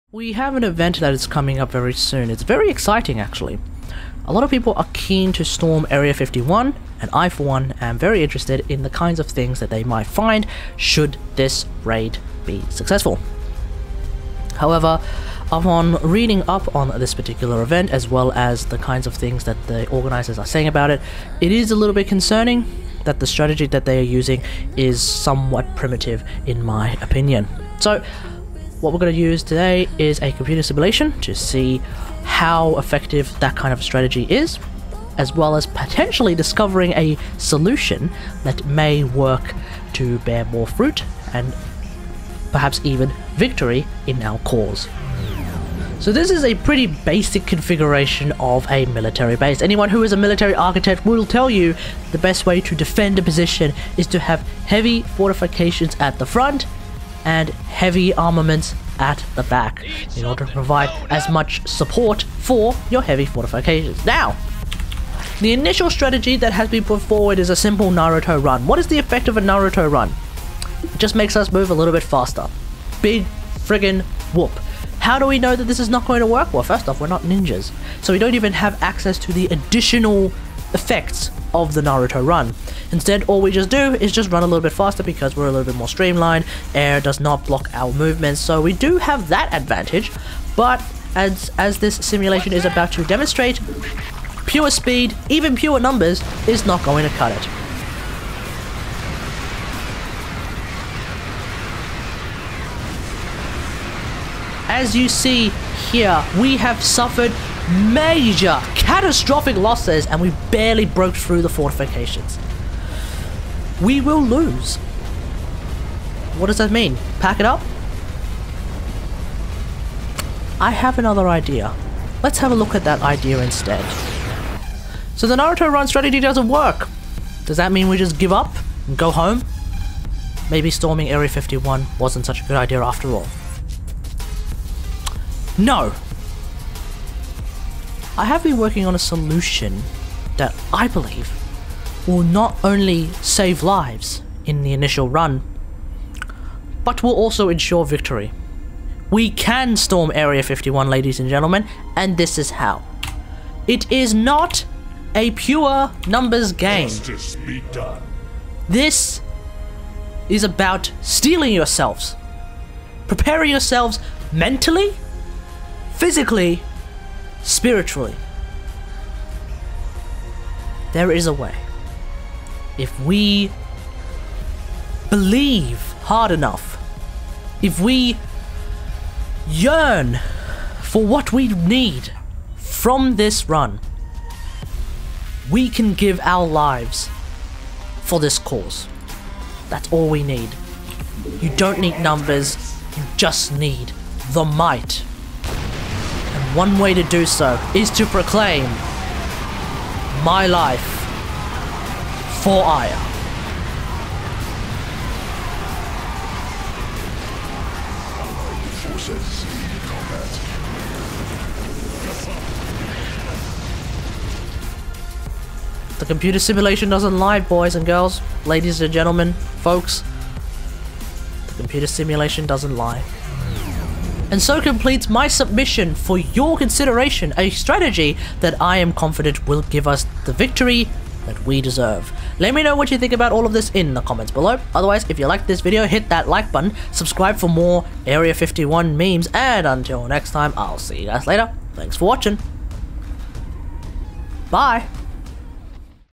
You CANNOT storm Area 51 with just a Naruto Run | Computer Simulation: My Life for Aiur
Today I wanted to push back on a MAJOR flaw that I see with the current strategy put forward by the organisers of September's event. I would like to see this raid succeed because I believe it's high time the people of the world learn of the deep dark secrets held within that shadowy clandestine place.